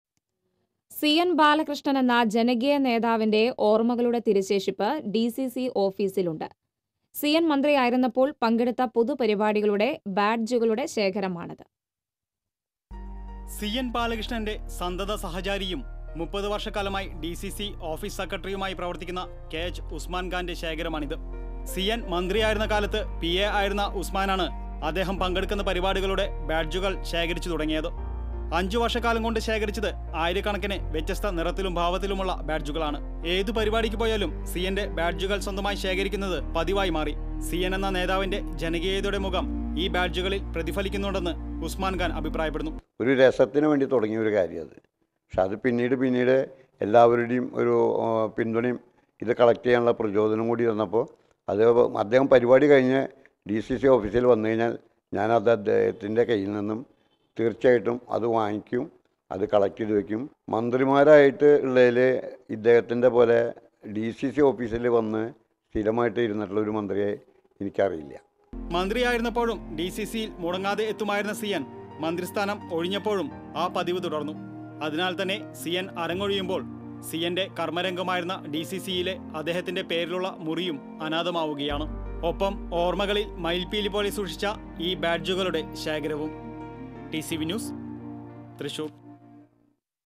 ജനകീയ നേതാവിന്റെ ഓര്‍മകൾ | TCV Thrissur
സി.എന്‍.ബാലകൃഷ്ണനെന്ന ജനകീയ നേതാവിന്റെ ഓര്‍മകളുടെ തിരുശേഷിപ്പ് ഡിസിസി ഓഫീസിലുണ്ട്. സി.എന്‍. മന്ത്രിയായിരുന്നപ്പോള്‍ പങ്കെടുത്ത പൊതു പരിപാടികളുടെ ബാഡ്ജുകളുടെ ശേഖരമാണത്.